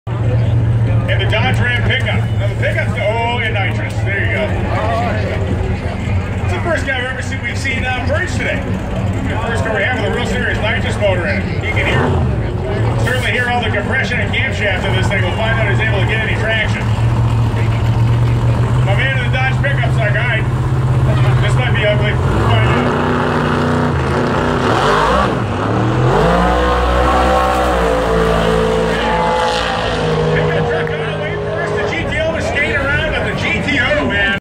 Roadkill drag races on Woodward part 3
GTO beats Pickup Truck!
Real Muscle Real Power Real Drag Race!